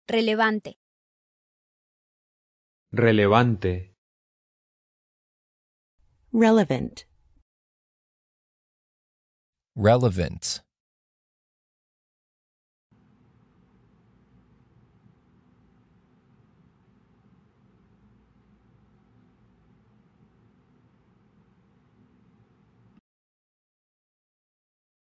How to say "Relevant" in Spanish
Male Voice:
Relevante

Female Voice:
Relevante

Male Voice Translation:
Relevant

Female Voice Translation:
Relevant